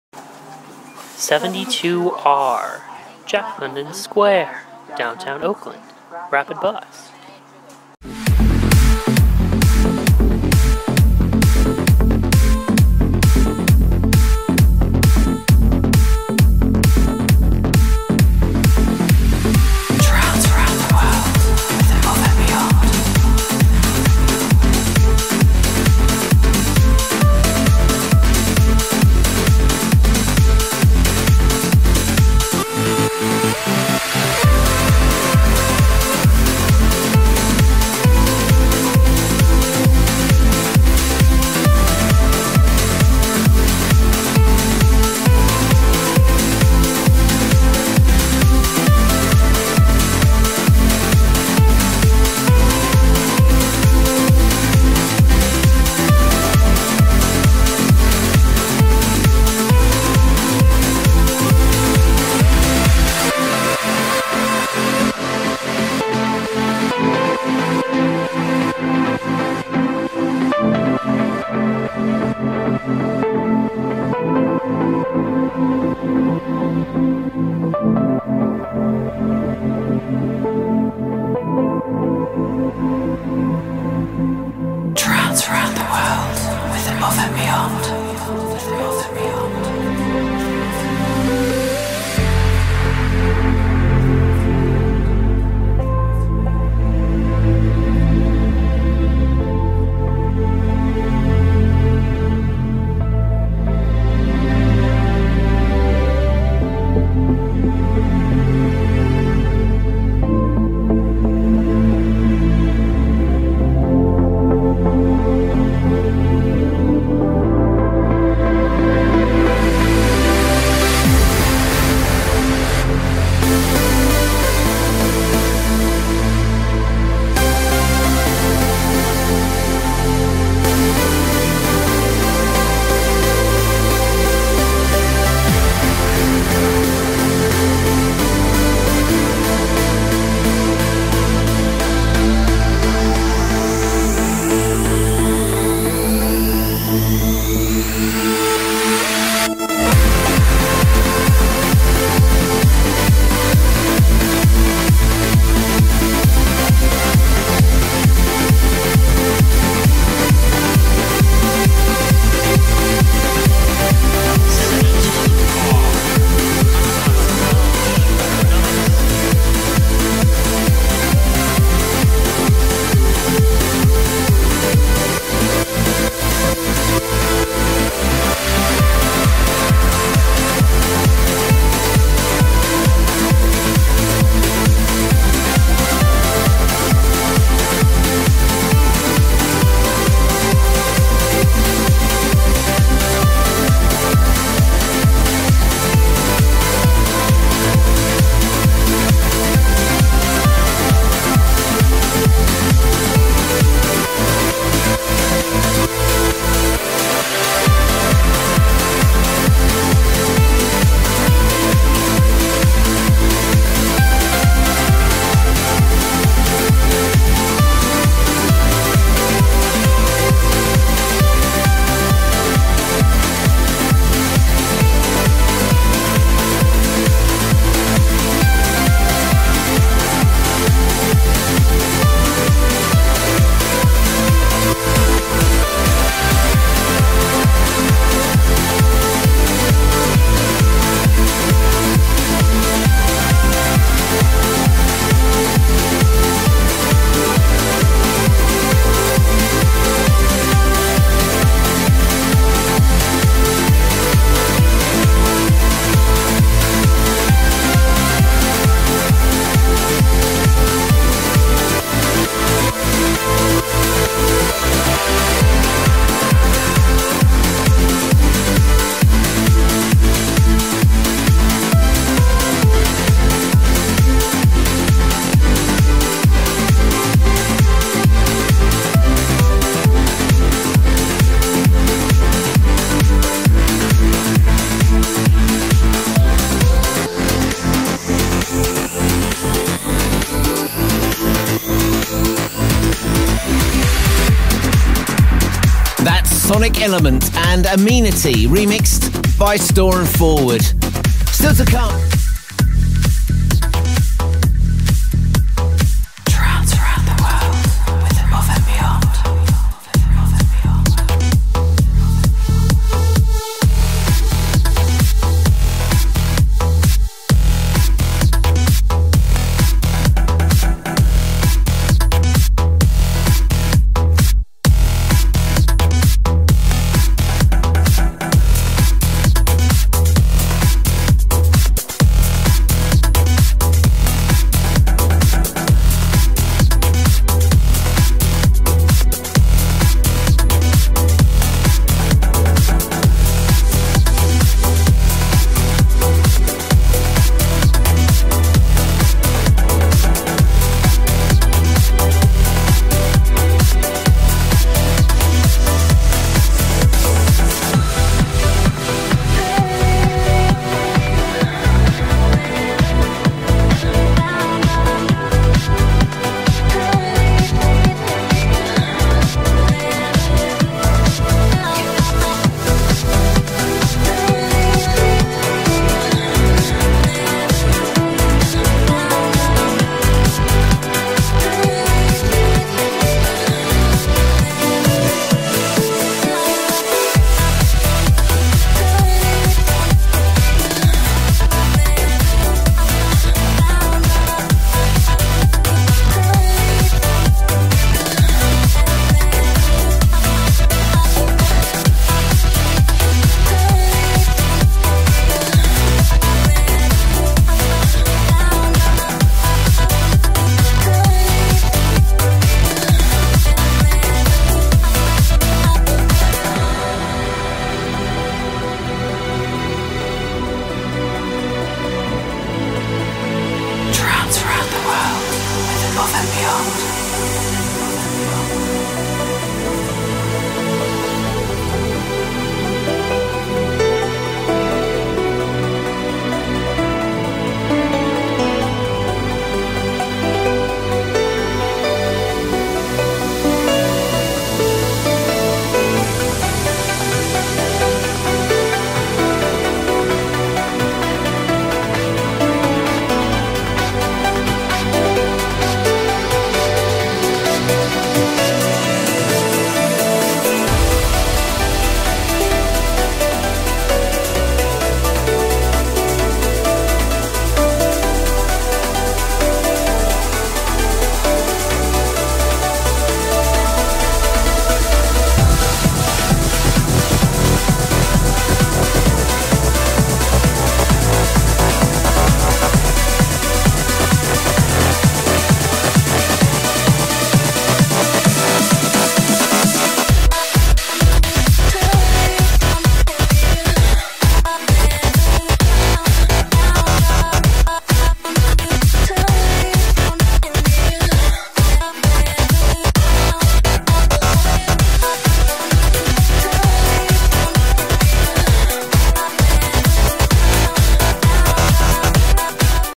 AC transit: 72R San Pablo Ave Rapid Bus
video of a ride on the 72R rapid bus to Contra costa college in san Pablo,  CA.
AC transit
This line travels roughly 20-40mph Top speed being 40mph.